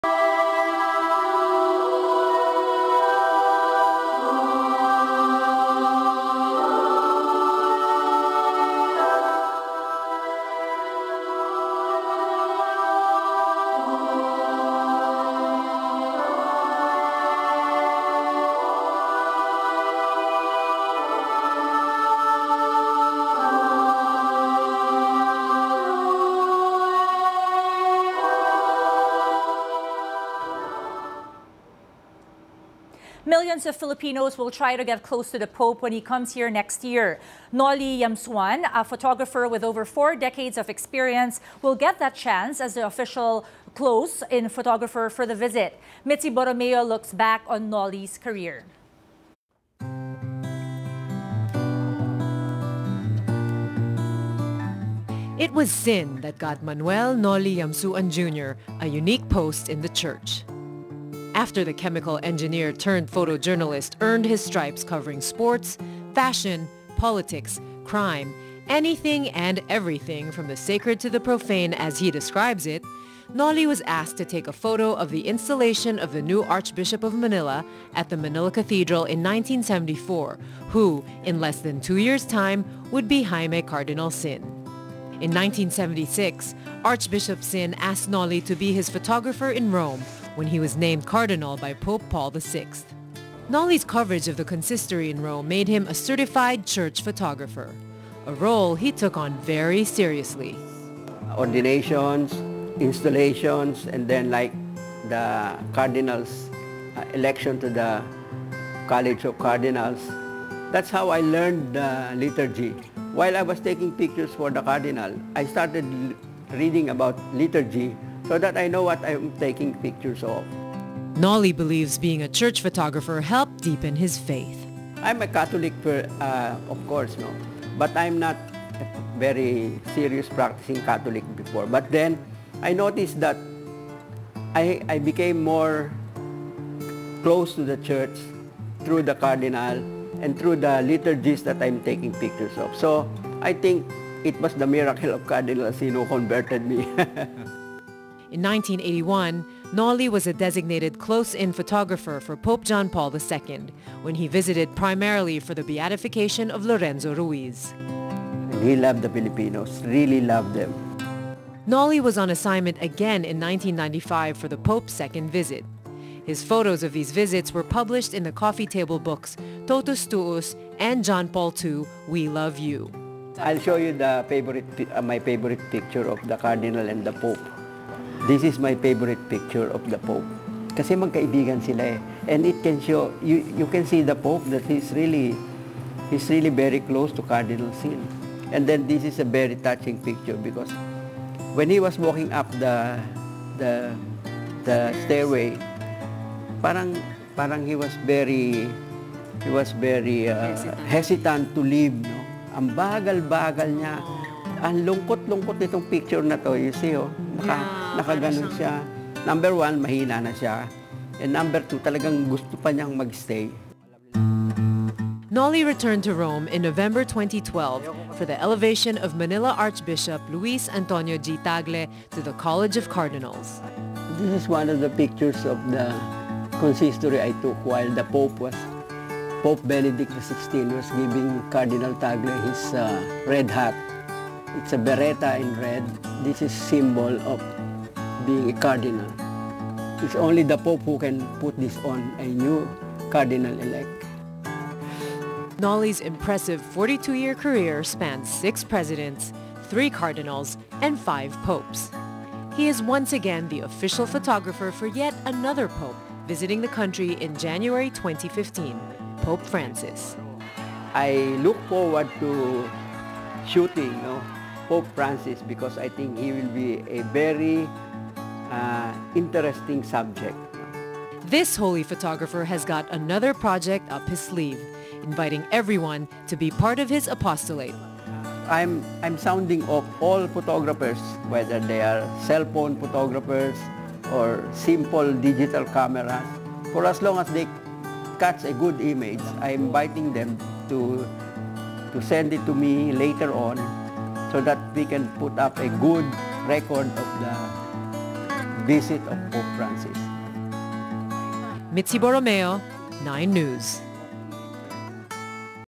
Pope Francis In The Philippines
Millions of Filipinos will try to get close to the pope when he visits the country in January.